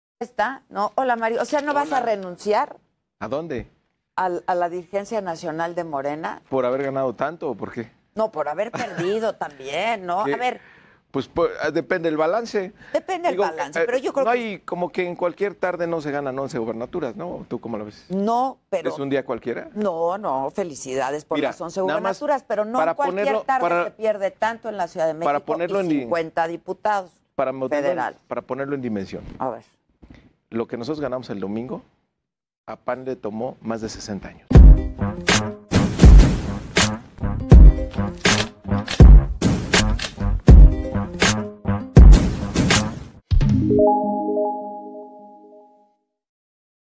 Así le contestó Mario Delgado a Adela Micha ¡Tómala!
Medios internan minimizar el triunfo de morena en las pasadas elecciones.
Así contestó Mario Delgado a Adela Micha.
"Lo que ganamos el domingo, el PAN tardó 60 años"
¡TÓMALA! 🤭
Crestomatía: @heraldot